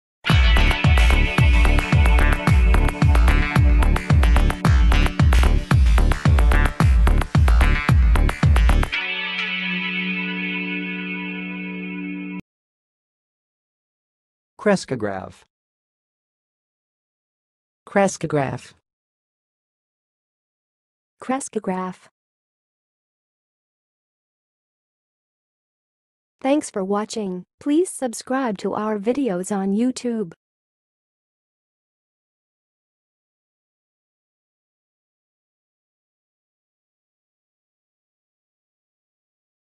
How to say "crescograph"! (High Quality Voices)
Watch in this video how to say and pronounce "crescograph"!